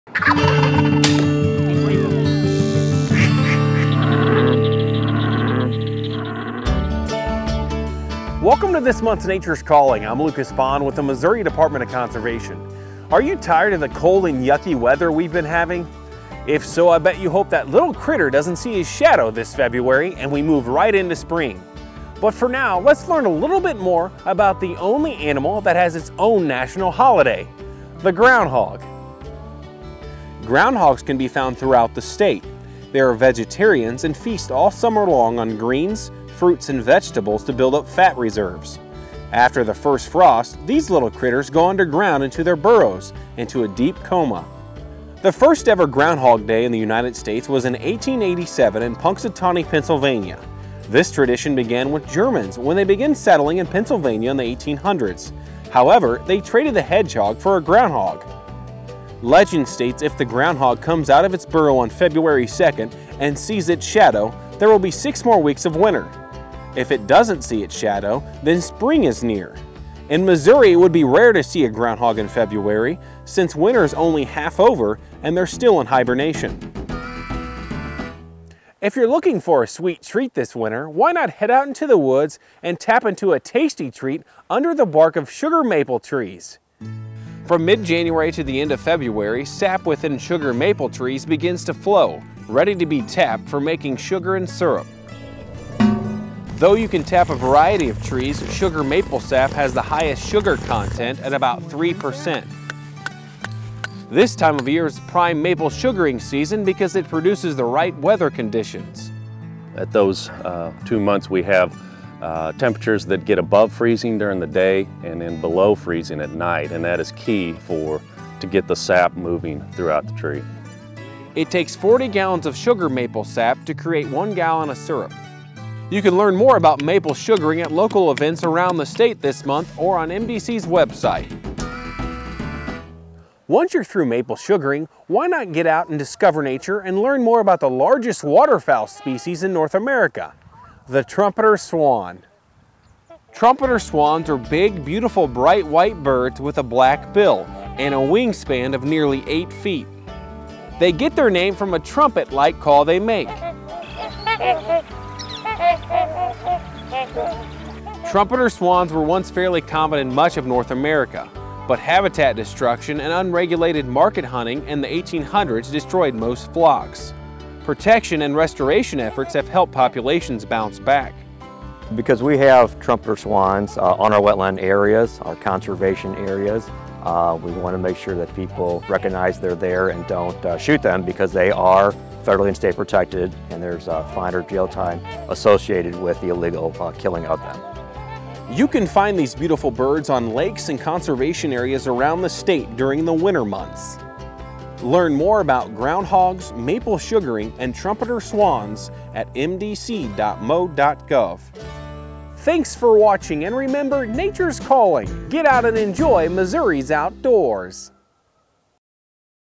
Nature's Calling - Groundhogs, Maple Sugaring, Swans (Feb 2021)
Have you ever wondered:
How did Groundhog Day get started?
How much maple sap does it take to make syrup?
Can you see swans in Missouri?
Get valuable info about Groundhogs, Maple Sugaring, and Trumpeter Swans in this monthly video segment.